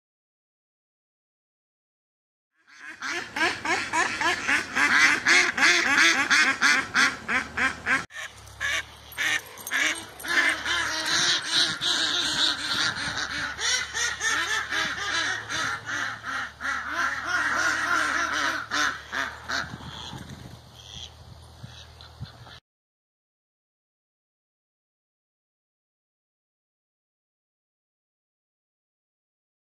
Quick flight around my house! #17
Here is a another quick flying video! Today Merry the mallard hen flew around my house! I really enjoy making flying videos, so there will probably be tons of flying videos in the future!